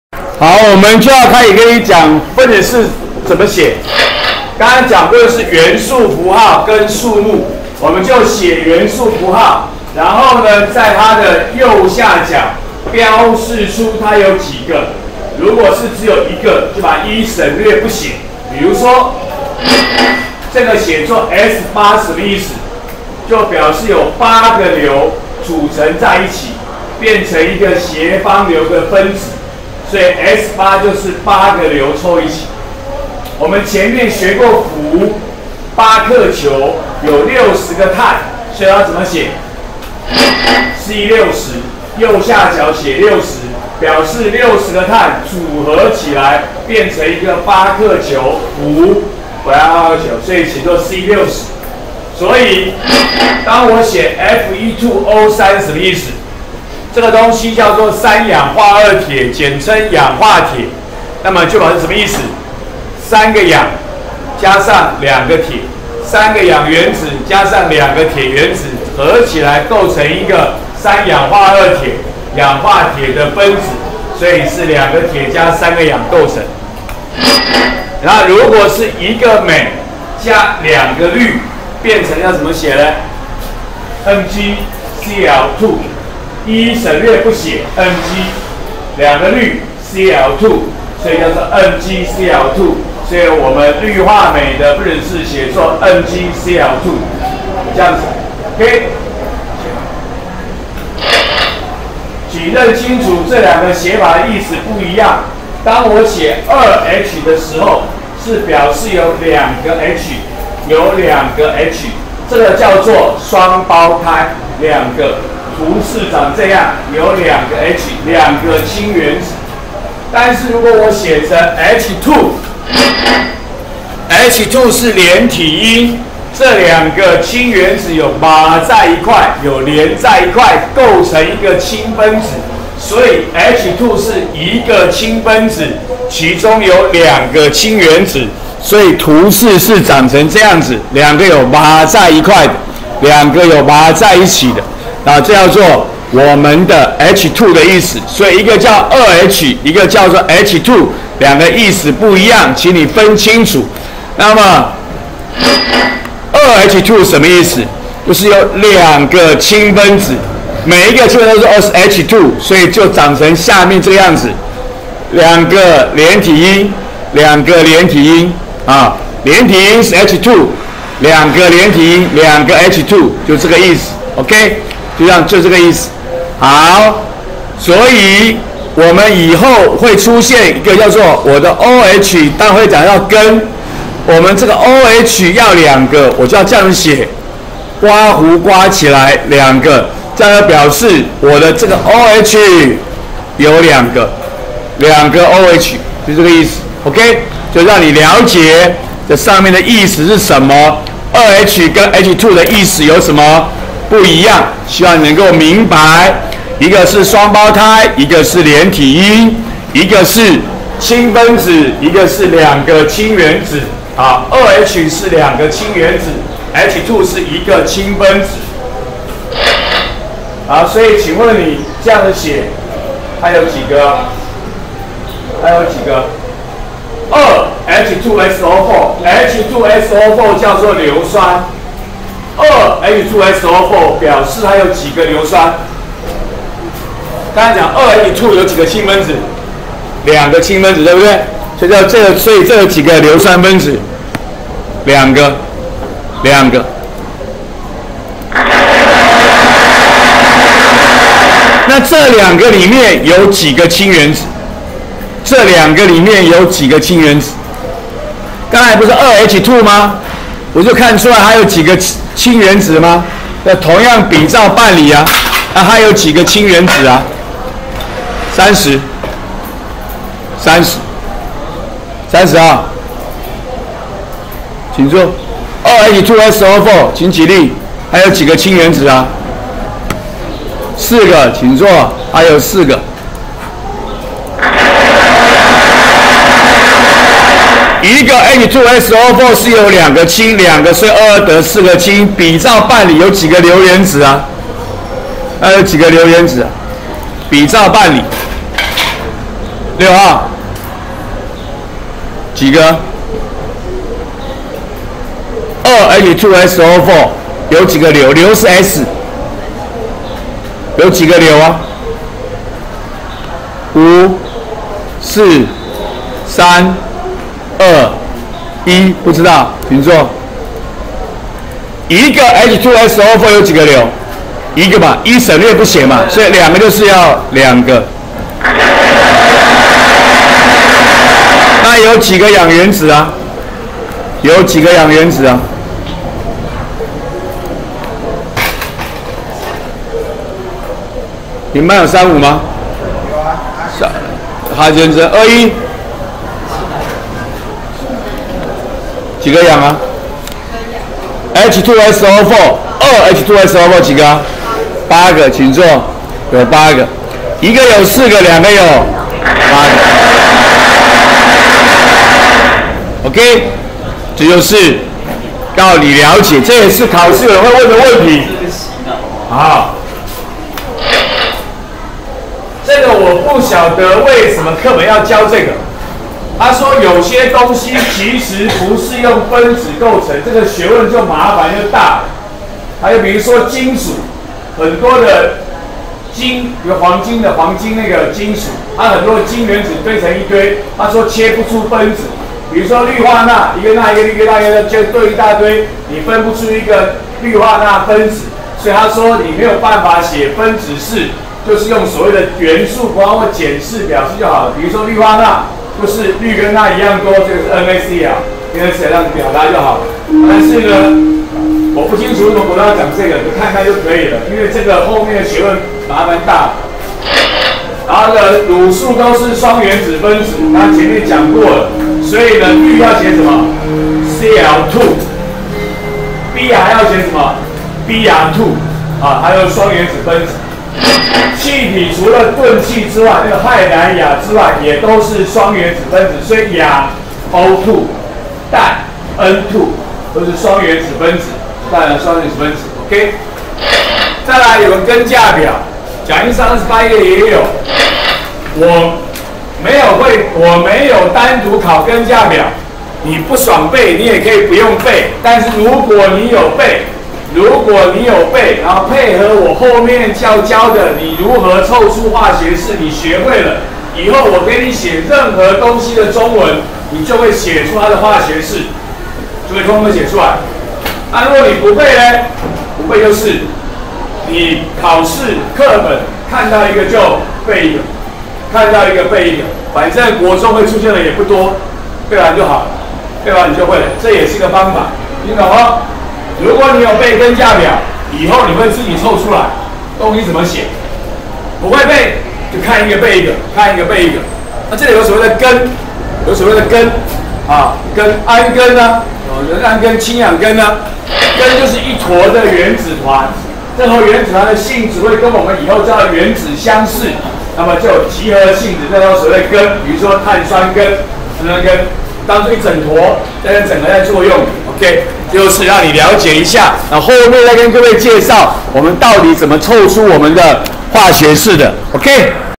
104學年國二理化＿元素與化合物12：分子式寫法、說明；根價表；根
說明分子式的意義，如何書寫，說明分子式內含的意義。介紹根價表及根。